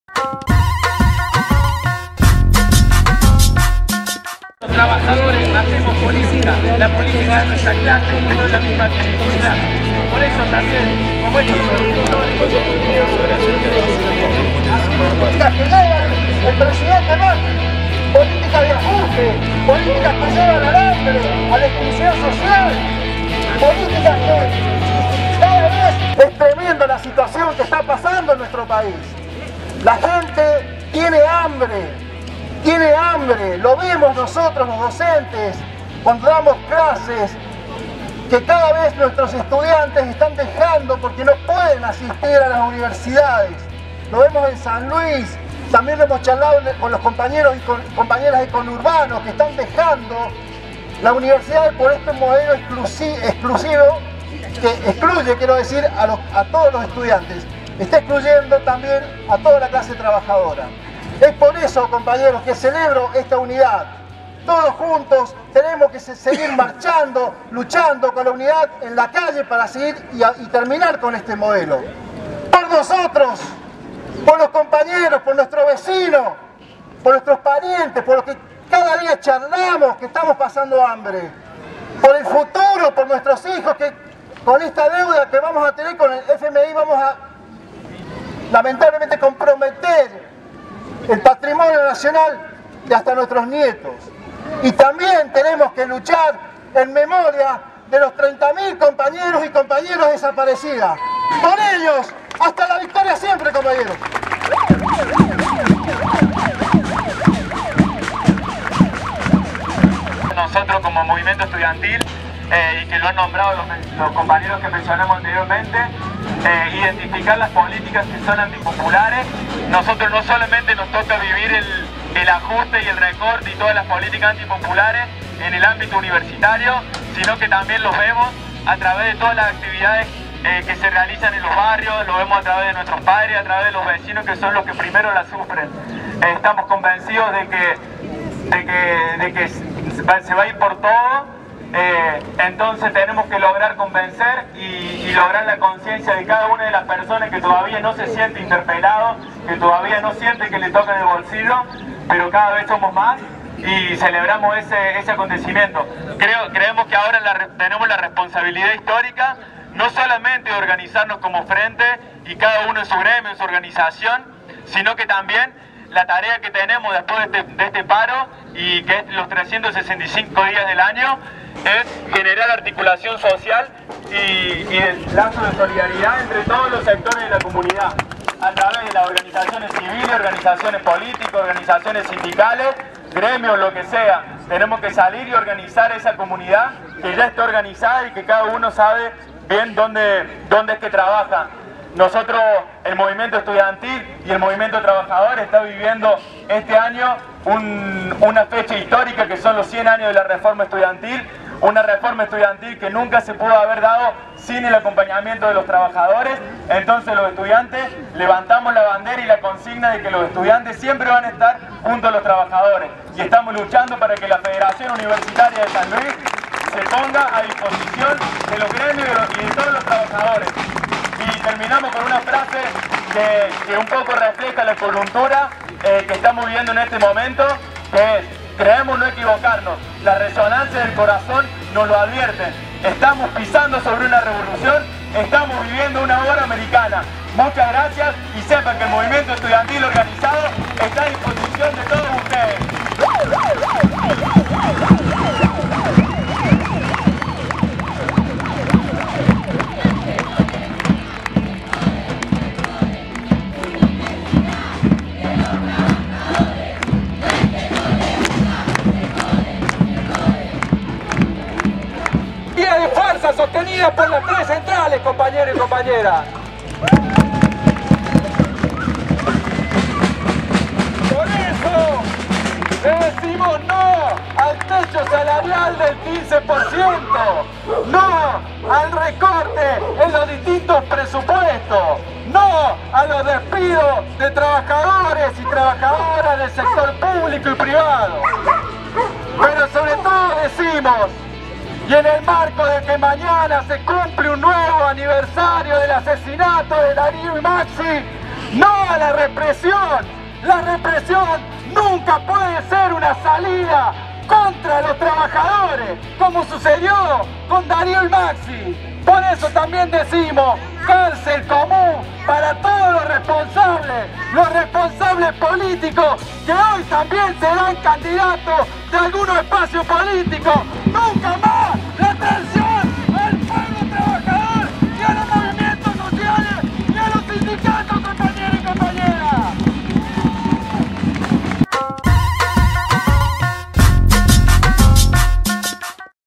ParoNacional
Marcha por el microcentro puntano por el ParoNacional en repudio al gobierno nacional.
La CTA Autónoma y las CTA de Los Trabajadores marchan nucleadas bajo un mismo reclamo: Decirle basta al ajuste macrista.
También marchan estudiantxs, mujeres feministas por el derecho al aborto, jubiladxs, partidos políticos y gremios de San Luis.